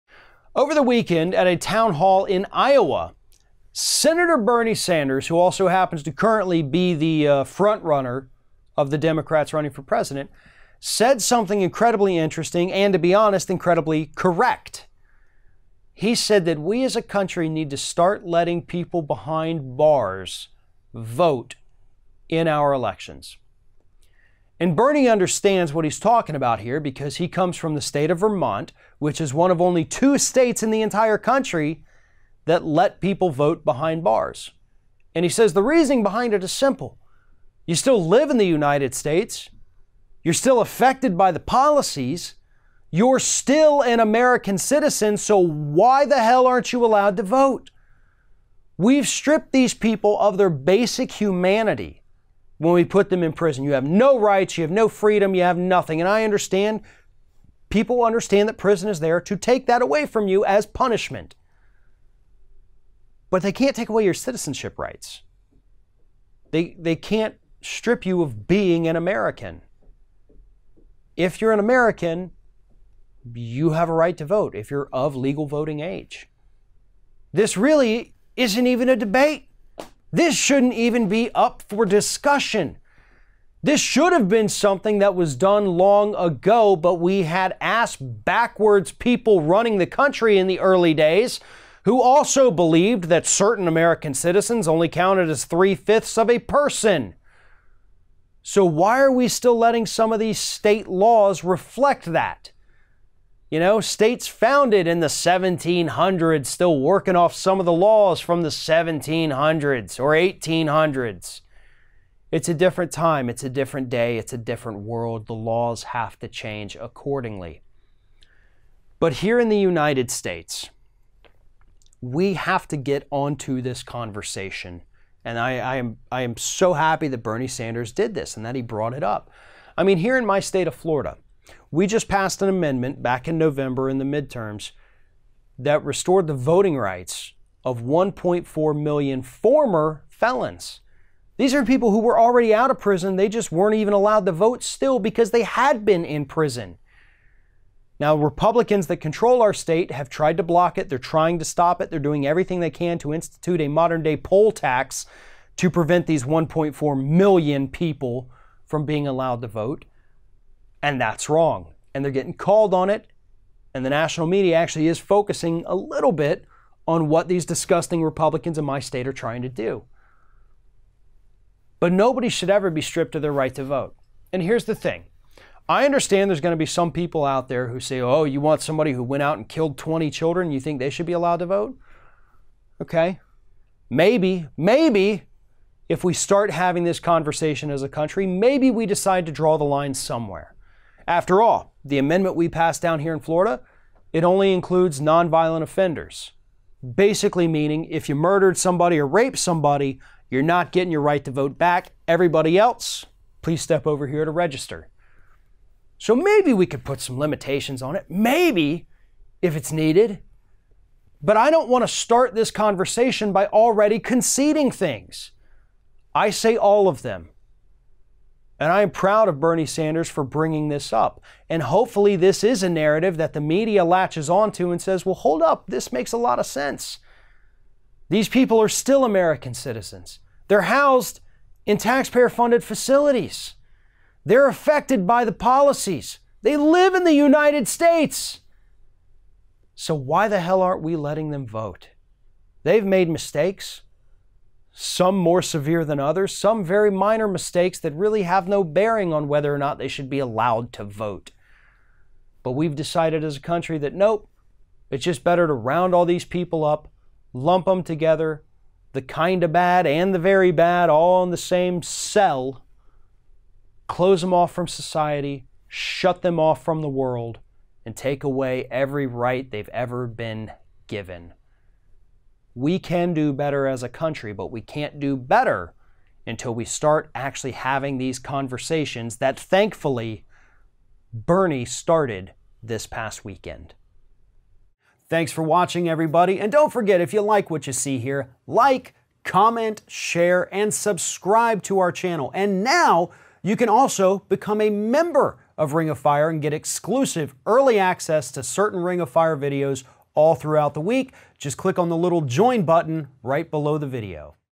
Bernie Is Right – Prisoners Should Be Allowed To Vote
The right to vote is something that we’re still trying to figure out in the United States, and that alone is a sad statement to have to make. But Senator Bernie Sanders wants to take another leap forward for voting rights by extending the right to vote to people in prison. They are still citizens, after all, and they still live in the country and are effected by the rules and decisions of politicians, so shouldn’t they also have a say? Ring of Fire’s Farron Cousins explains why this is a good idea.

Over the weekend at a town hall in Iowa. Senator Bernie Sanders, who also happens to currently be the front runner of the Democrats running for president, said something incredibly interesting and to be honest, incredibly correct. He said that we as a country need to start letting people behind bars vote in our elections and Bernie understands what he's talking about here because he comes from the state of Vermont, which is one of only two states in the entire country that let people vote behind bars. And he says the reason behind her to simple, you still live in the United States, you're still affected by the policies. Your still an American citizen. So why the hell aren't you allowed to vote? We've stripped these people have their basic humanity when we put them in prison. You have no rights, you have no freedom, you have nothing. And I understand people understand that prisoners there to take that away from you as punishment, but they can't take away your citizenship rights. They can't strip you of being an American

if you're an American, you have a right to vote. If you're of legal voting age, this really isn't even a debate. This shouldn't even be up for discussion. This should have been something that was done long ago, but we had asked backwards people running the country in the early days who also believed that certain American citizens only counted as three fifths of a person. So why are we still letting some of these state laws reflect that? You know, states founded in the 1700 still working off some of the laws from the 17 hundreds or 18 hundreds it's a different time. It's a different day. It's a different world. The laws have to change accordingly. But here in the United States, we have to get on to this conversation and I am, I am so happy that Bernie Sanders did this and that he brought it up. I mean, here in my state of Florida, we just passed an amendment back in November in the midterms that restored the voting rights of 1.4 million former felons. These are people who were already out of prison. They just weren't even allowed. The vote's still because they had been in prison. Now, Republicans that control our state have tried to block it. They're trying to stop it. They're doing everything they can to institute a Modern Day poll tax to prevent these 1.4 million people from being allowed to vote, and that's wrong and they're getting called on it. And the national media actually is focusing a little bit on what these disgusting Republicans in my state are trying to do.